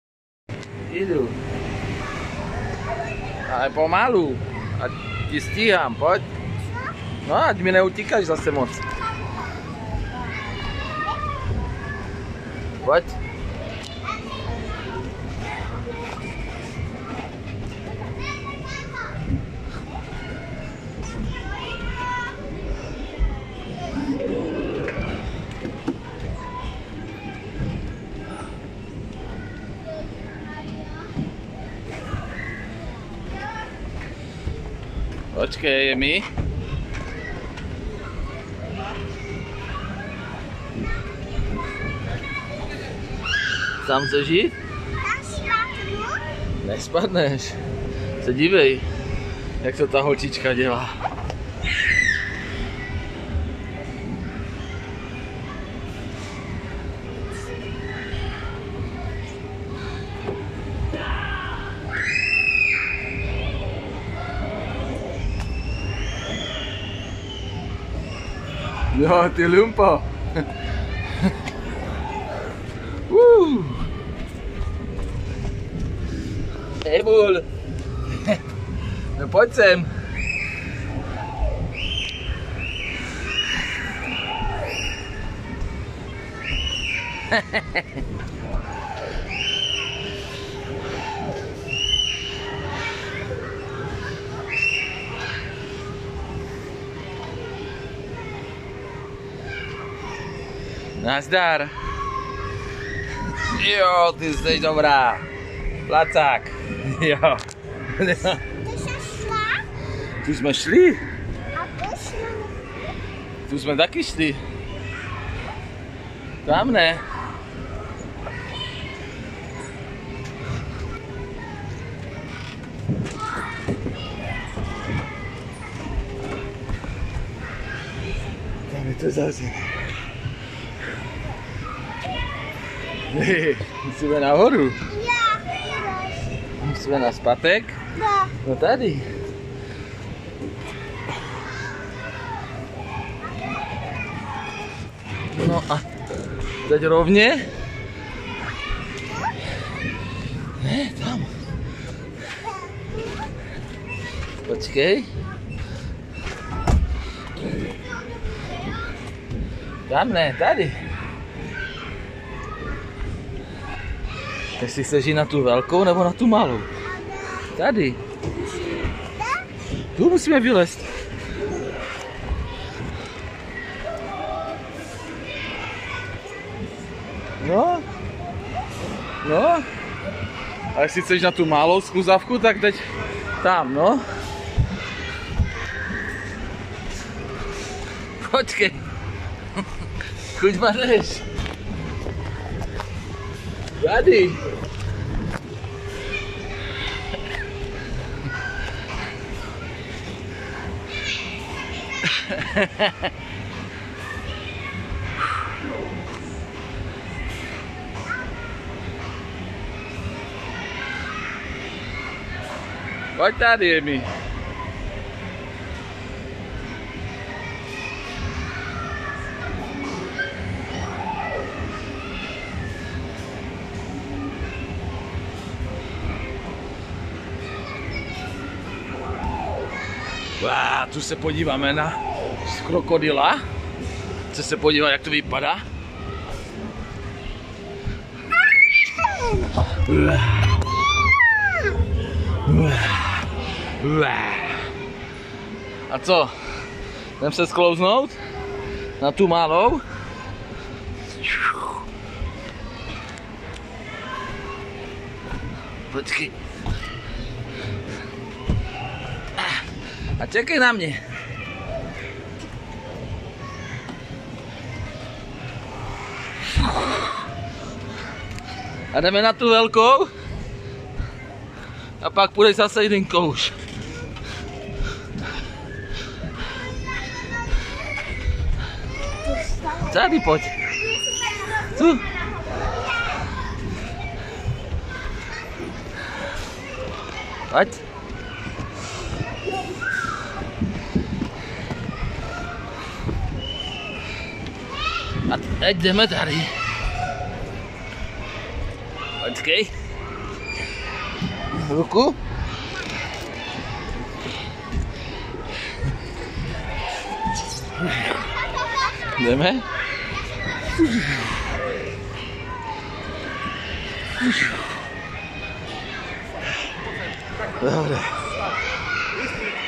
FunPark Žirafa Ostrava/Emma🦒🐣 návod jak zničit taťku 😁
Tak jsme si trošku zasportovali 😉😁🐣v tomhle skvělém relax prostředí... kde zažijete spousty zábavy s vašíma ratolestma... Užívejte 😉👍